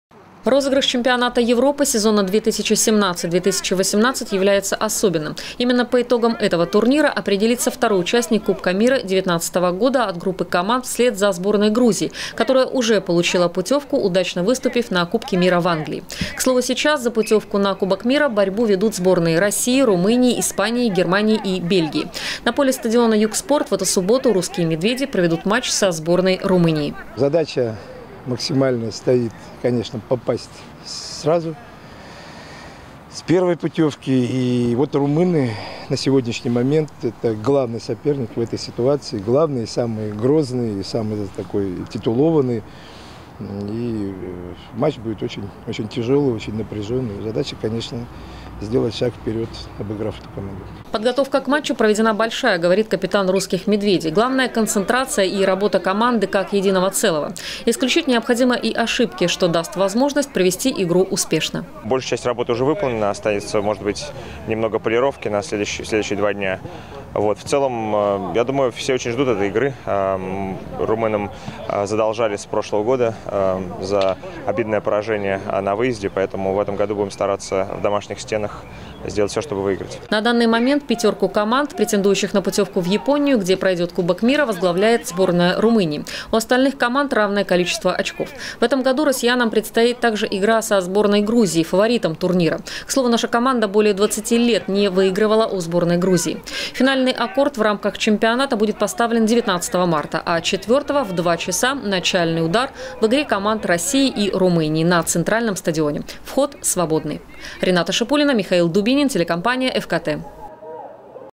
В Сочи пройдёт матч по регби. Новости Сочи Эфкате
В субботу на центральном стадионе «Юг Спорта» пройдёт матч чемпионата Европы по регби среди мужских команд между сборными России и Румынии. Это квалификационные отборочные игры, которые обеспечивают путёвку на кубок мира 2019. В преддверии матча наша команда провела открытую тренировку.
Сочи. Новости.
Телеканал Эфкате.